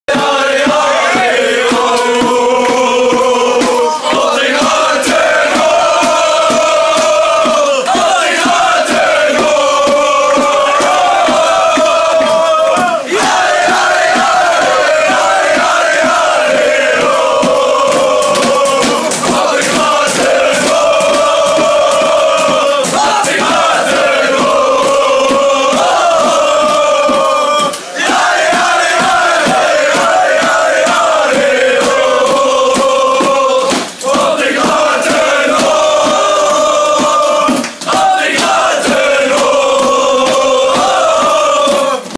Fans des FSV Optik Rathenow feiern
In der Regionalliga Nord-Ost spielt der FSV Optik Rathenow gegen die zweite Mannschaft von Union Berlin. Die Rathenow-Fans feiern auf dem Weg nach Berlin.